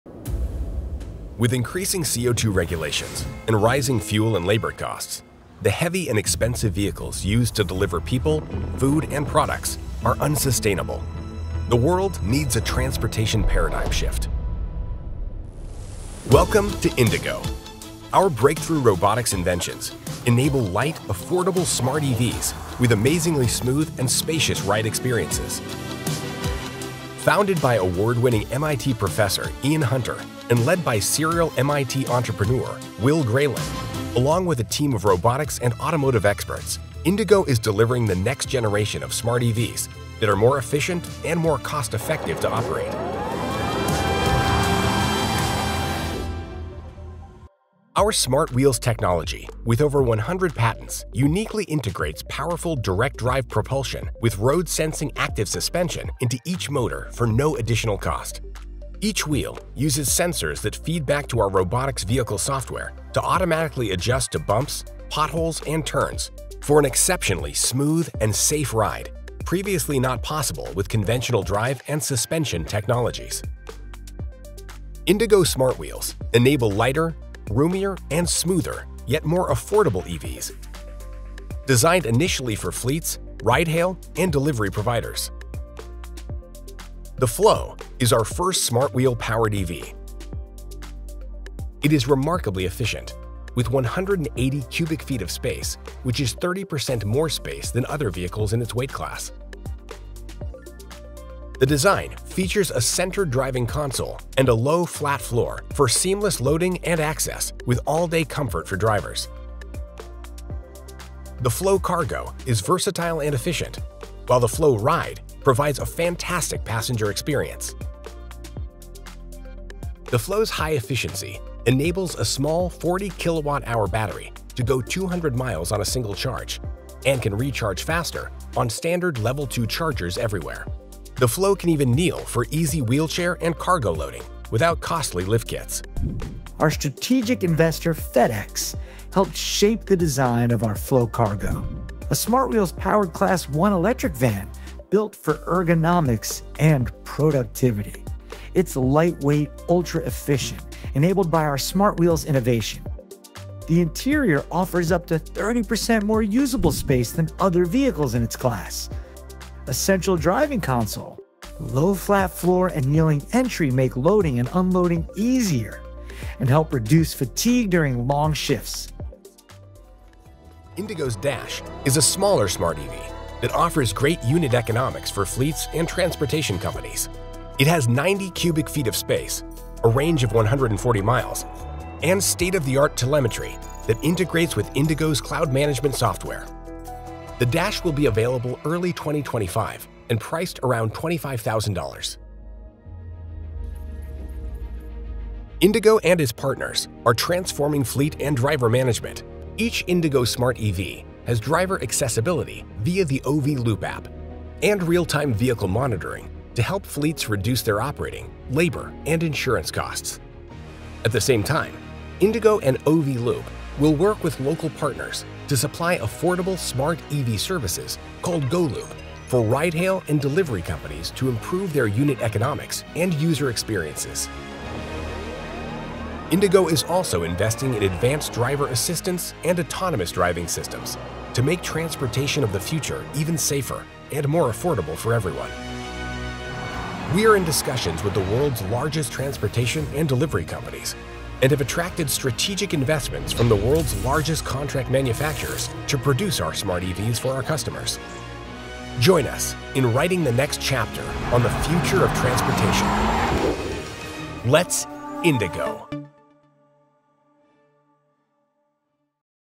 Meet indiGO | Redesigning Vehicle Architecture for Smarter EVs
indiGO Technologies is a Boston-based electric vehicle company building affordable EVs purpose-built for fleets, ride-hail, and delivery. Backed by FedEx, Foxconn, and FM Capital, indiGO is reshaping fleet electrification with vehicles that are roomier, more efficient, and ready for real-world use.

Our patented SmartWheels™ technology integrates propulsion, suspension, and sensing into each wheel, enabling a new vehicle architecture that delivers a smoother ride, improved ergonomics, and better energy performance.

The Flow, our first vehicle, features a low flat floor, central driving position, and 180 cubic feet of interior space—30% more than others in its class. It offers wheelchair accessibility, a 200-mile range, and fast Level 2 charging.

The Dash is a compact, fleet-ready EV with cloud-based telemetry and smart fleet integration, providing 90 cubic feet of cargo space and a 140-mile range.

Let’s indiGO!♾️